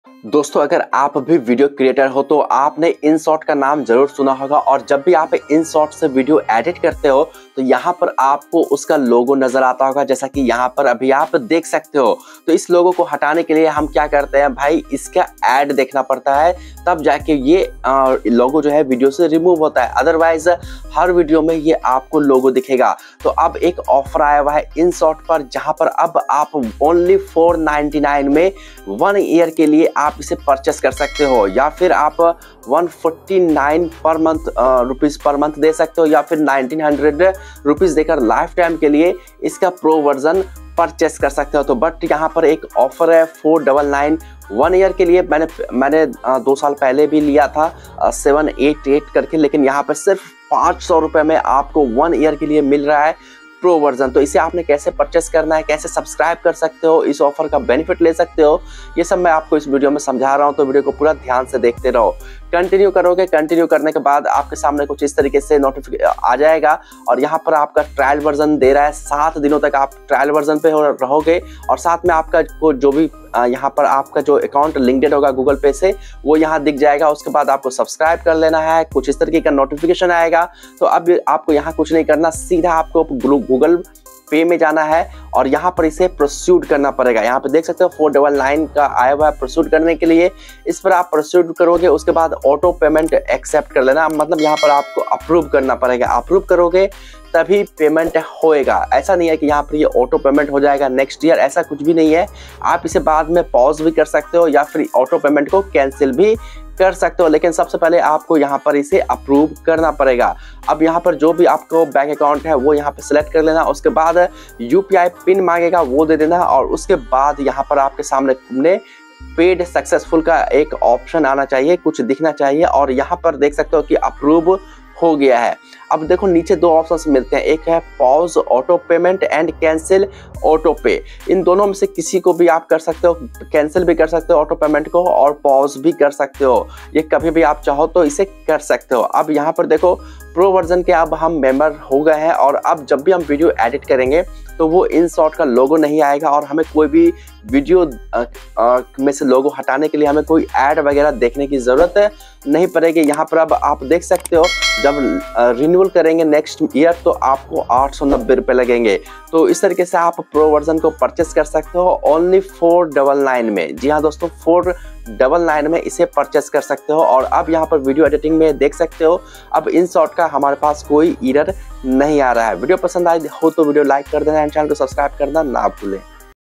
Inshot Big Sale Offer - 50% off one year plan only 499 Rupees | Inshot Pro Plan 50% Off
⚫ Install And Earn Money From Home ⚫

🛑 Open Your Demat Account For Trading Free 📌

📌 5 Paisa Download & Install 📌

🛑 Callme4 I'd :- trickyazim ( Quick Support )